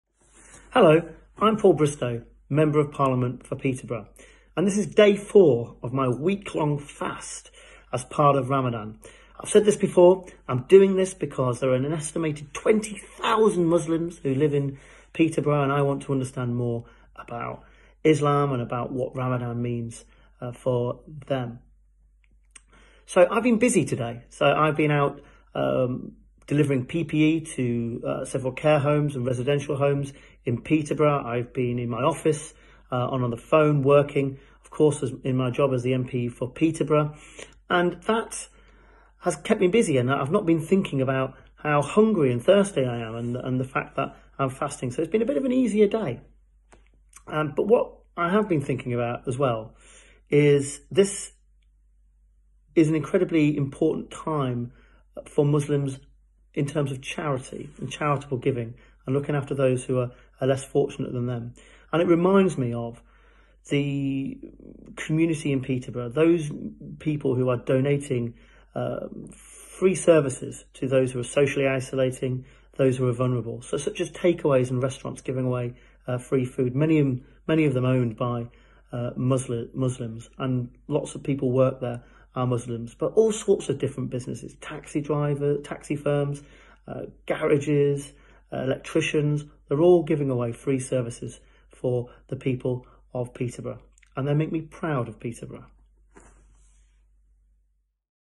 Ramadan fasting vlog day 4
A busy day whilst fasting at Ramadan in Peterborough